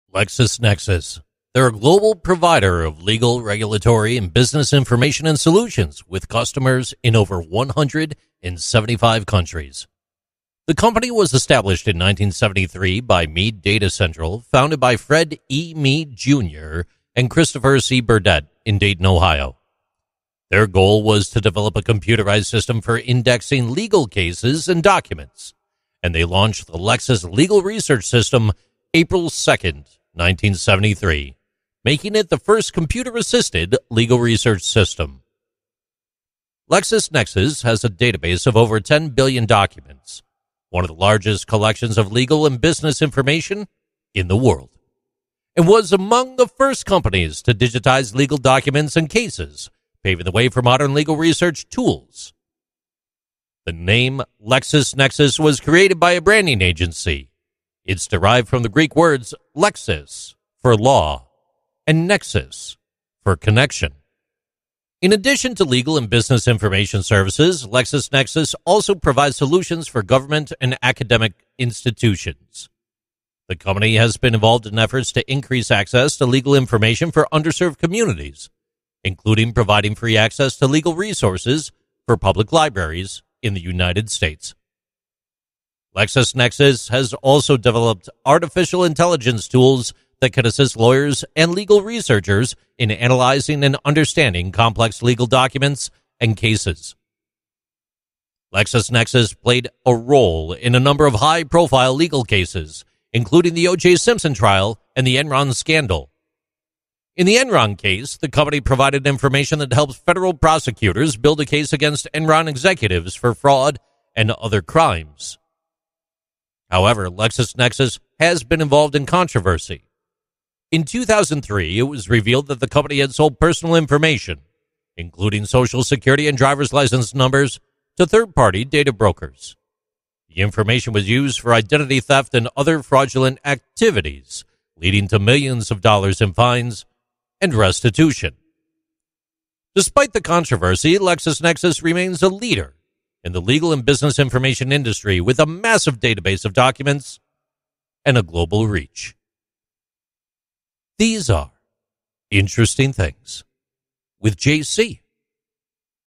605: LexisNexis
Interesting Things with JC #605: "LexisNexis" launched April 2nd 1973. Discover the fascinating history of legal research and business information service, LexisNexis.

From revolutionizing legal research to playing a role in high-profile cases, LexisNexis has been a game-changer.